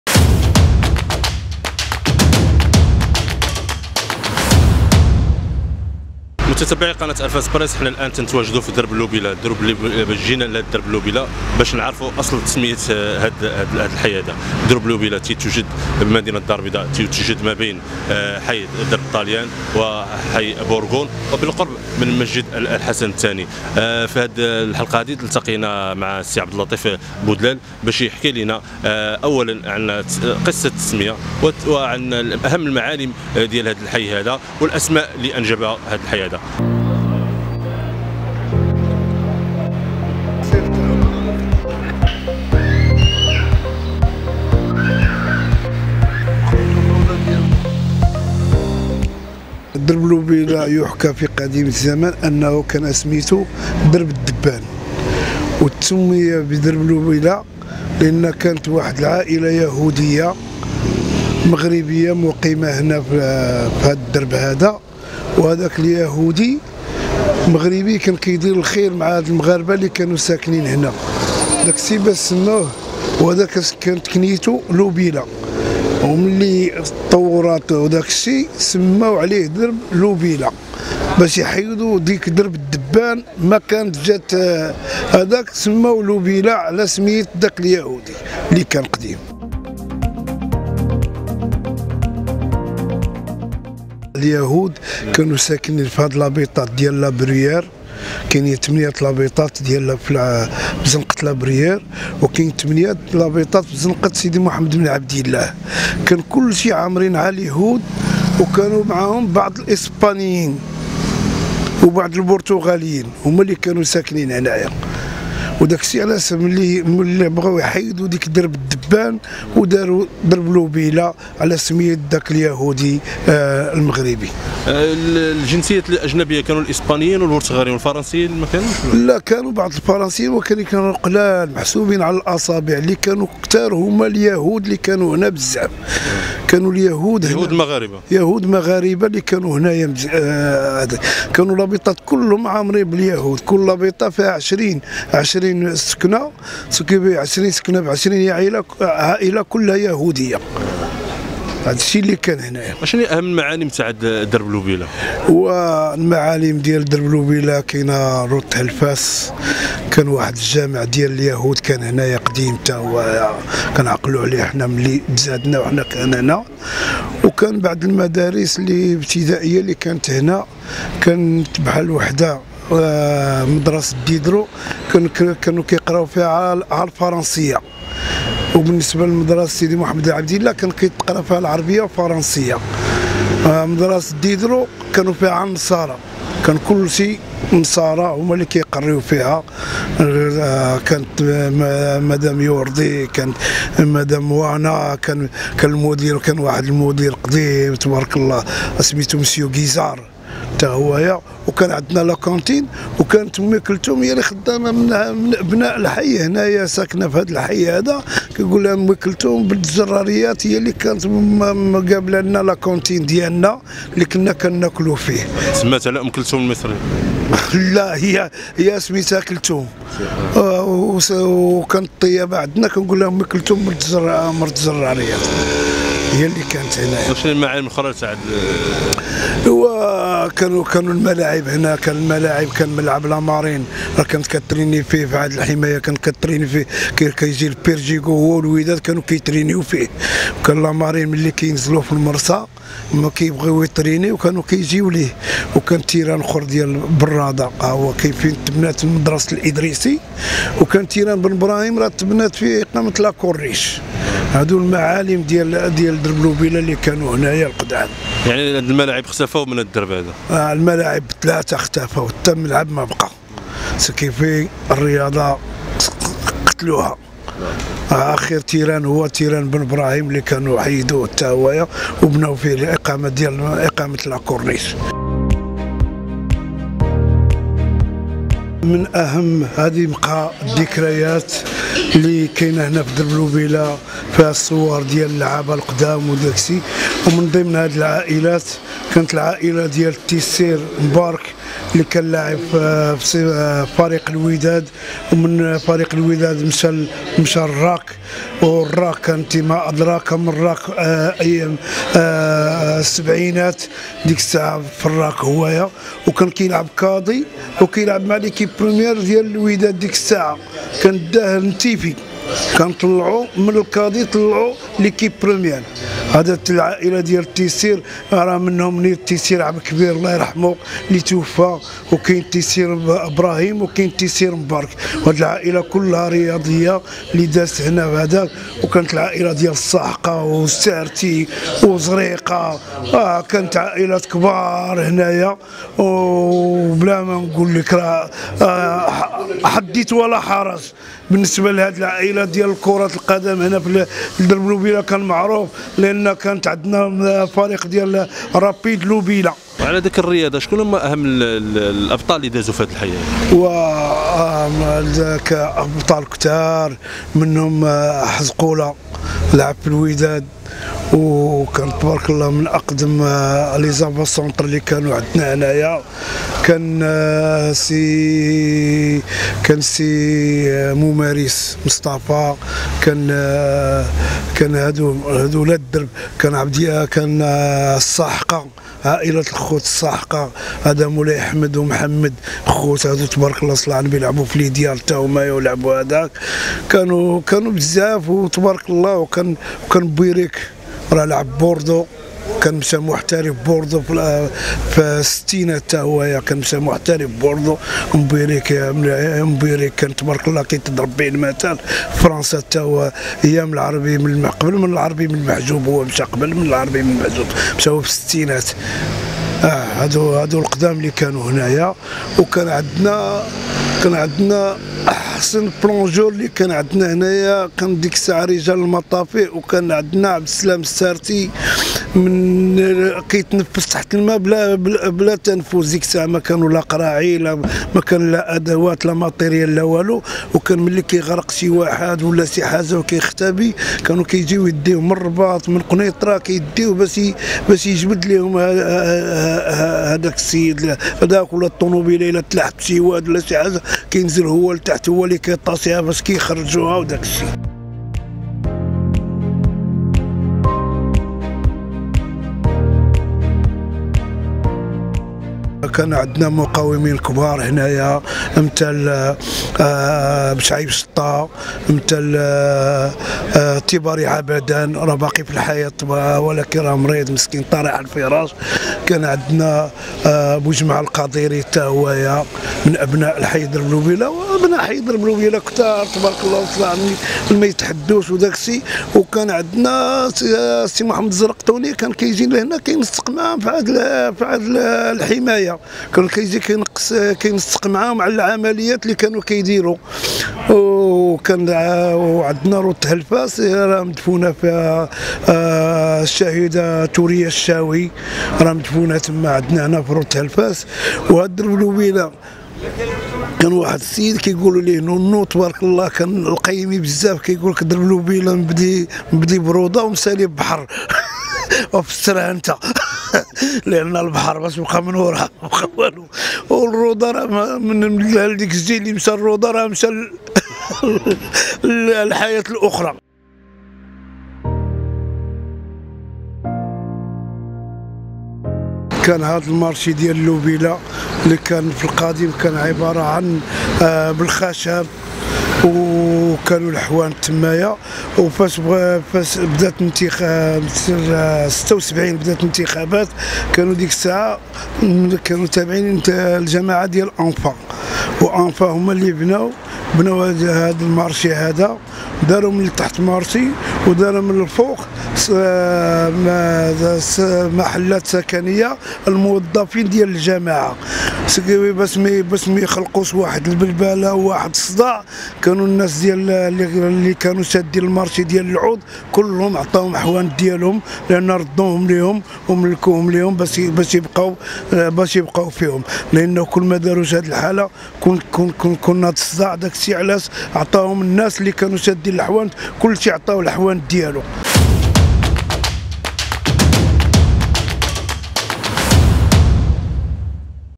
لوبيلا.. اليهودي الذي منح اسمه لأشهر حي بكازا
واحد من أبناء حي لوبيلا بالدارالبيضاء يحكي أصل تسمية هذا الحي الذي يعد واحد من أشهر الأحياء في المدينة القديمة. درب لوبيلا في كازا يوجد بين درب الطالبان وحي بوركون

أخبار المغرب
# أخبار عاجلة